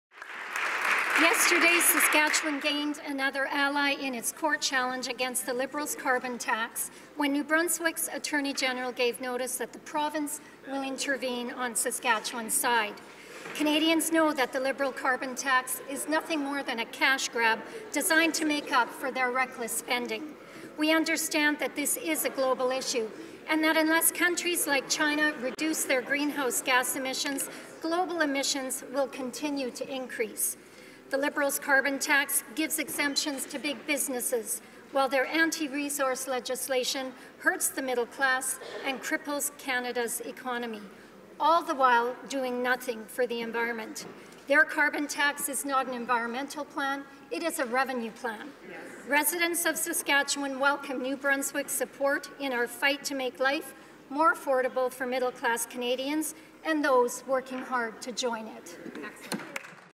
MP Kelly Block - New Brunswick joins Saskatchewan in fight against Carbon Tax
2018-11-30

MP Kelly Block
Member of Parliament for Carlton Trail - Eagle Creek